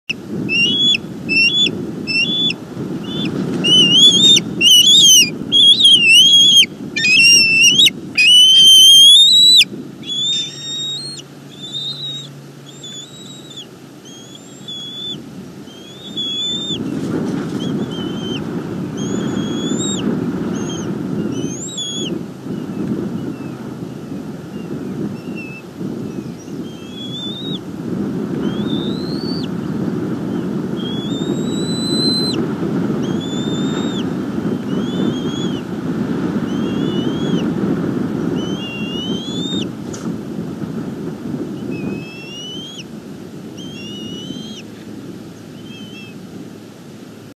Disappointment / NEFL Cam 1 / 2022-02-27 / 14:23
NE 27 goes to get the food brought. It turns out to be a tuft of grass :(